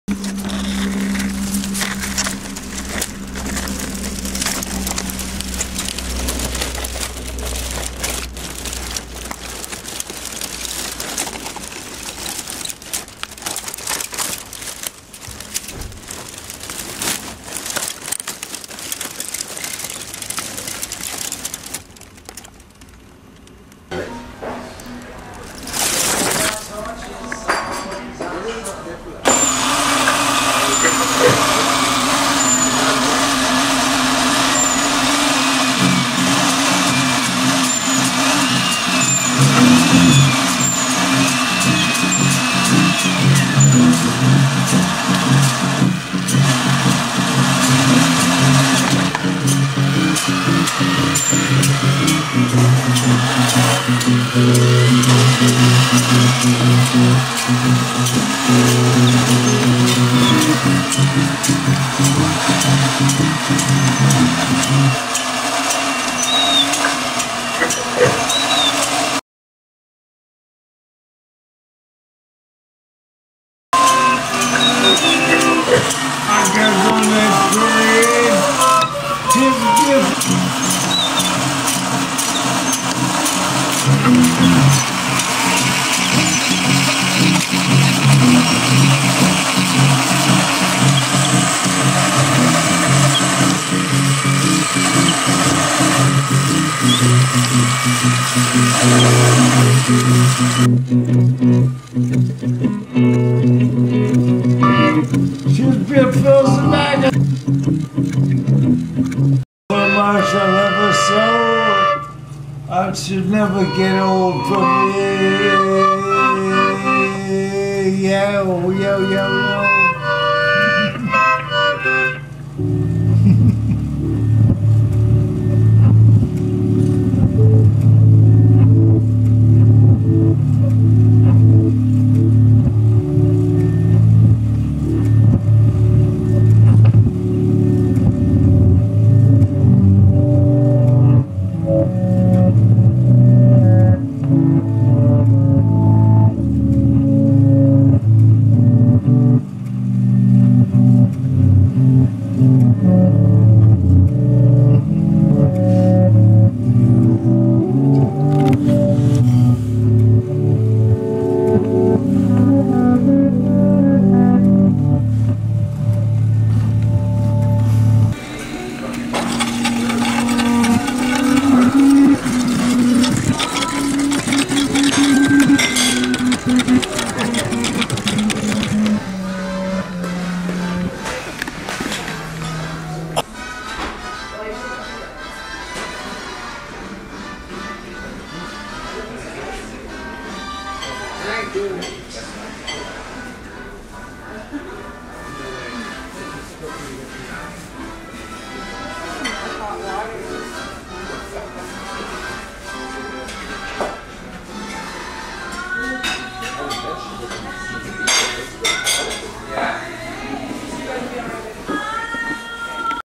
Time Traveler's Notebook Workshop
Time Traveler's Notebook
Workshop

Price:  Free
Place: Video Pool
Sundays Thru- February 7:00 PM - 10:00 PM
Screening in March 2017
3rd floor
100 Arthur Street
Winnipeg
Ed Ackerman and Gregory Zbitnew
A Workshopped movie based on the book "Time Traveler's Notebook"  by Gregory Zbitnew

Workshopped - Definition:
Present a performance of (a dramatic work), using intensive group discussion and improvisation in order to explore aspects of the production prior to formal staging:
‘the play was workshopped briefly at the Shaw Festival’... in this case it will be a movie presentation.